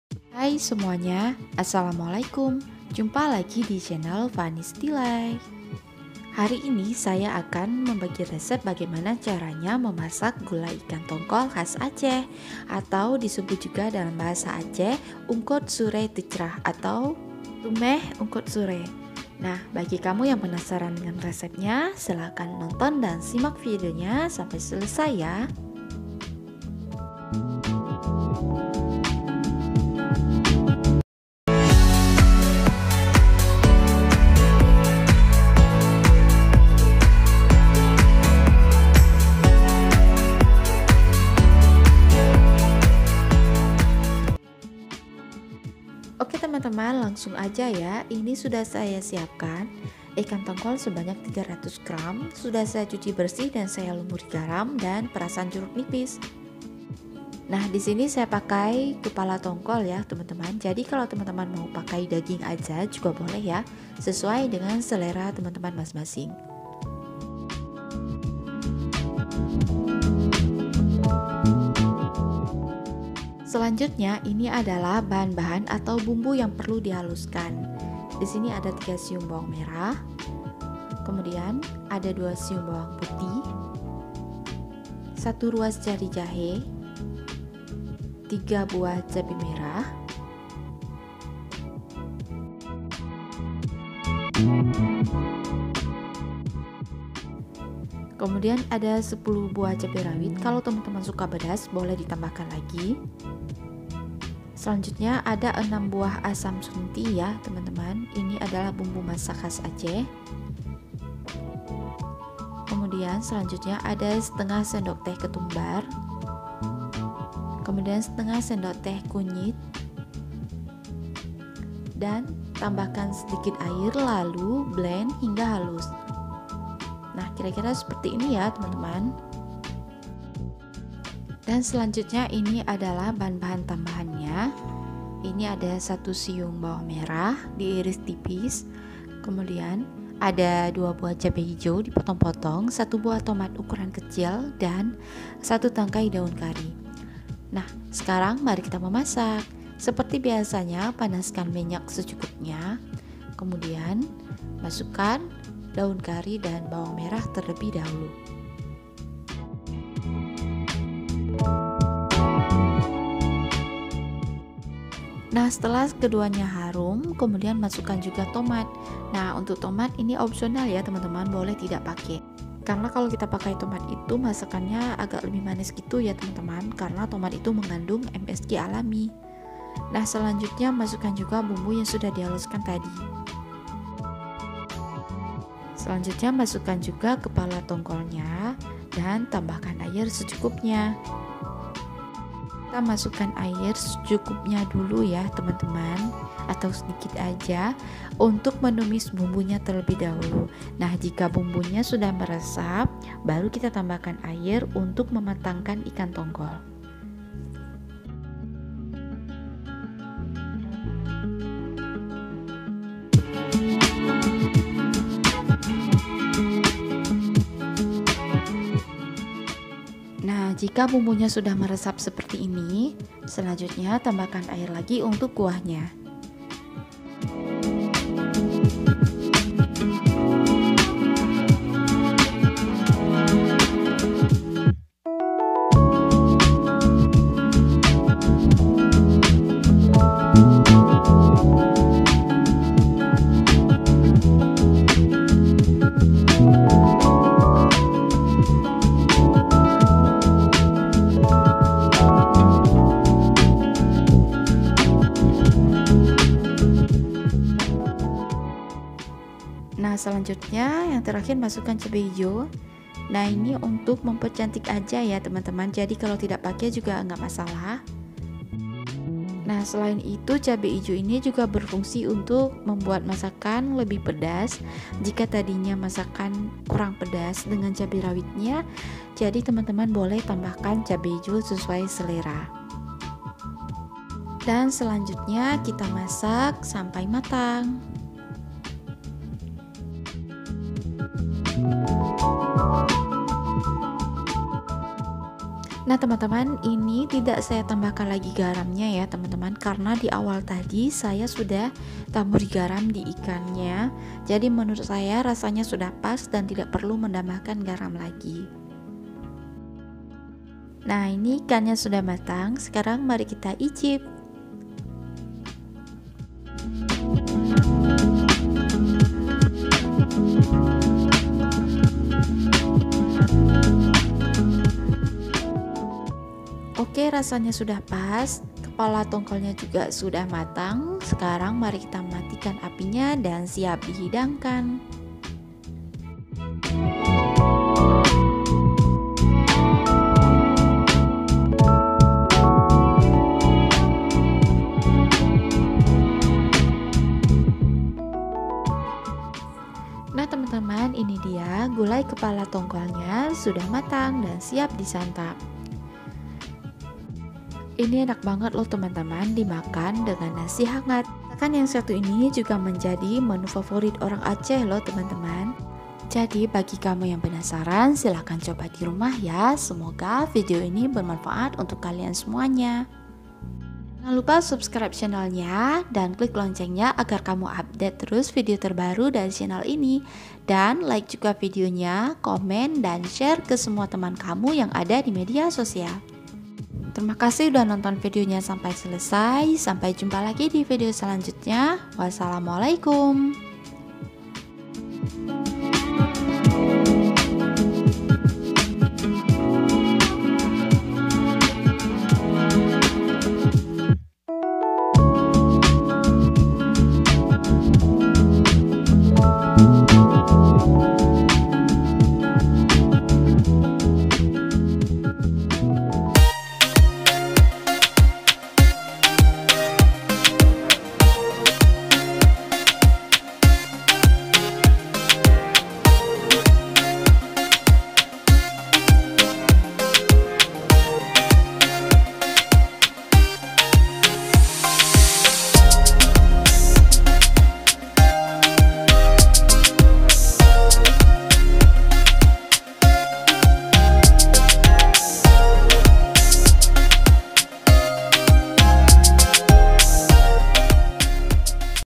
Gulai kepala ikan tongkol khas Aceh
hai sahabat fani's delight...
gimana kabarnya hari ini? sudah punya ide untuk memasak ? kalau belum yuk kita masak gulai kepala ikan tongkol, tapi ini masakan aceh ya gaes.. gulai ikan tongkol atau sering disebut dengan "unkot sure teucrah" ini sangat enak disantap dengan nasi hangat loh.rasanya yang pas dengan lidah orang aceh membuat masakan yang satu ini menjdi masakan andalan keluarga di Aceh.
nah bagi teman2 di luar aceh yang gak punya asam sunti boleh di ganti dengan asam jawa ya, dengan cara larutkan ke dalam air terlebih dahulu. selamat mencoba ya semoga bermanfaat..  see u next time..  salam sukses selalu.

bahan:
300gr ikan tongkol (kepala)
3 siung bawmer
2 siung bawput
1 ruas jari jahe
3 buah cabmer
10 buah rawit
6 buah asam sunti
1/2 sdt ketumbar
1/2 sdt kunyit
garam scukupnya

bahan tambahan:
1 siung bawang merah
2 buah cabe ijo
1 buah tomat kecil
1 tangkal daun kari